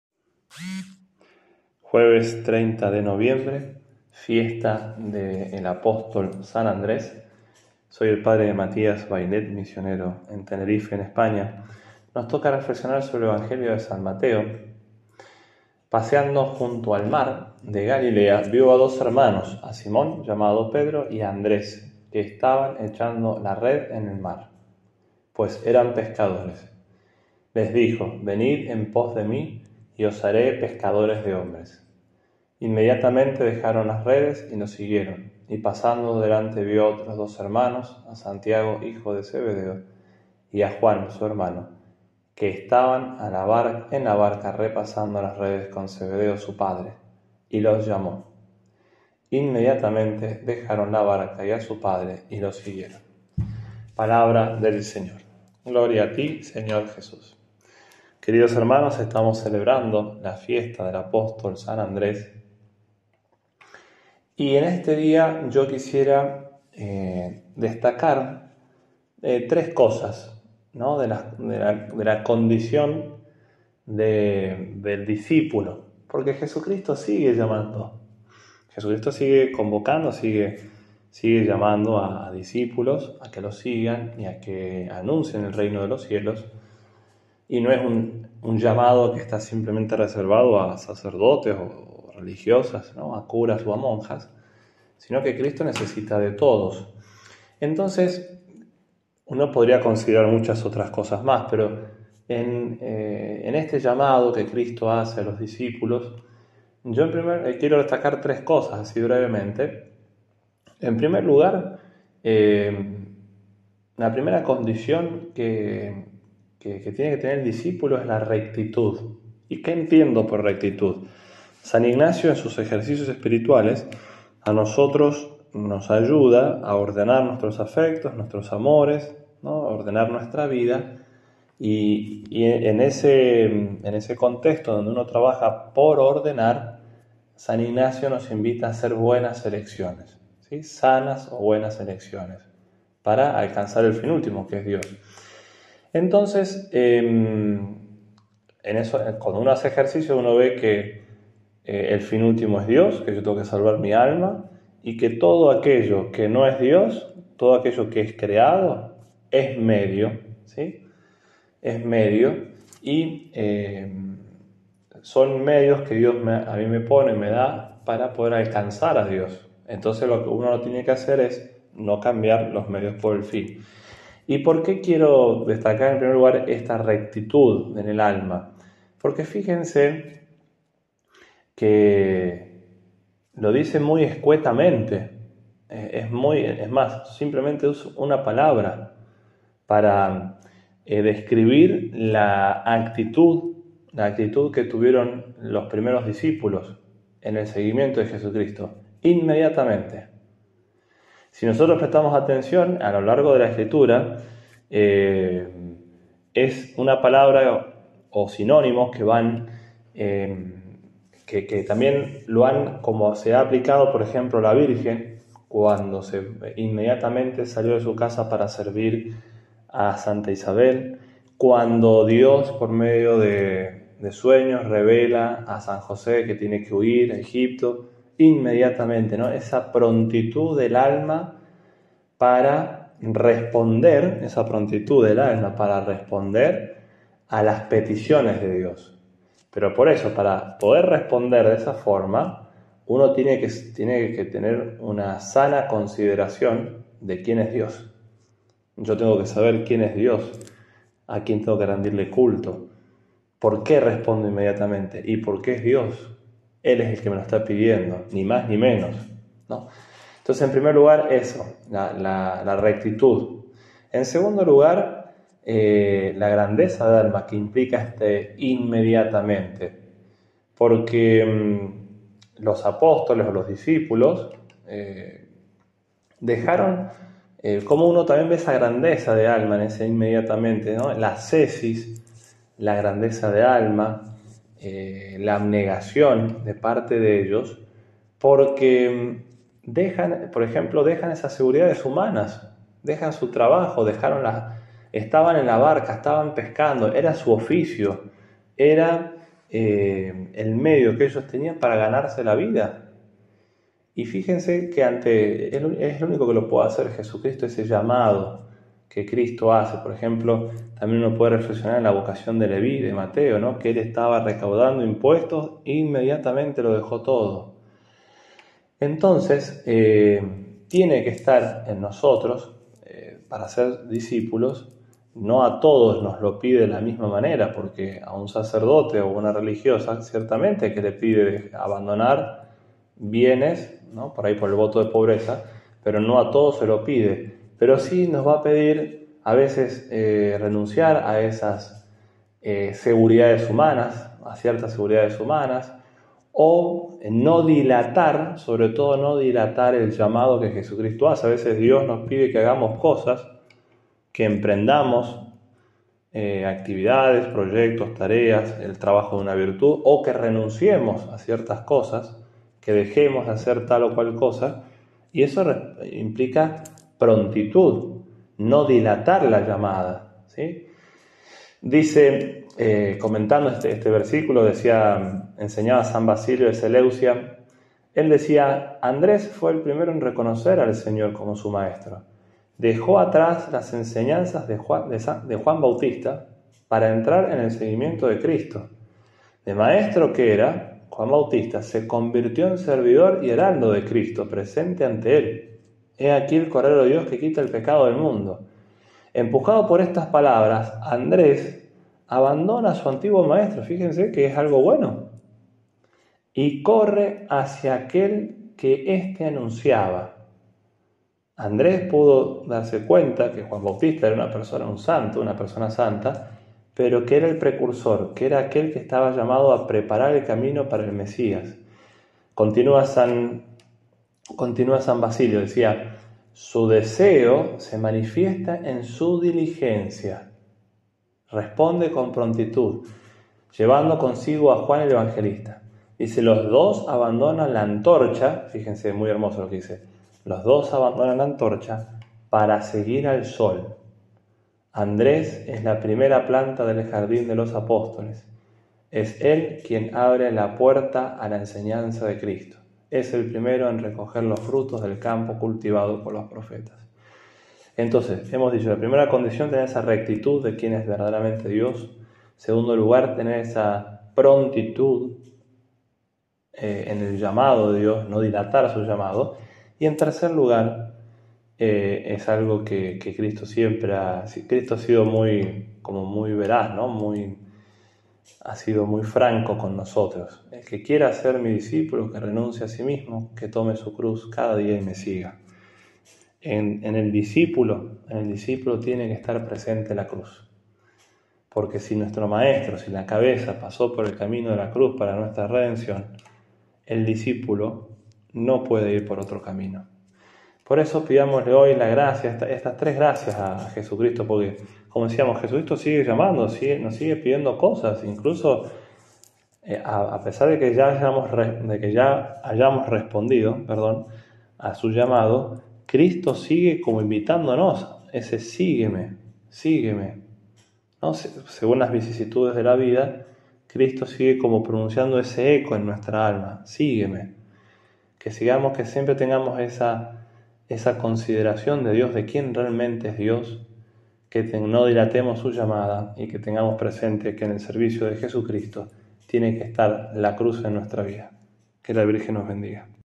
Evangelio de hoy, 30 de noviembre de 2023 | Cristo sigue llamando
Fiesta de San Andrés Apóstol
San Mateo
Paseando junto al mar de Galilea vio a dos hermanos: a Simón, llamado Pedro, y a Andrés, su hermano, que estaban echando la red en el mar, pues eran pescadores. Les dijo: “Venid en pos de mí y os haré pescadores de hombres”. Inmediatamente dejaron las redes y lo siguieron. Y pasando delante vio a otros dos hermanos; a Santiago, hijo de Zebedeo, y a Juan, su hermano, que estaban en la barca repasando las redes con Zebedeo, su padre, y los llamó. Inmediatamente dejaron la barca y a su padre y lo siguieron.

Si quiere colaborar con este Apostolado lo puede hacer dirigiéndose a . Desde ya muchas gracias.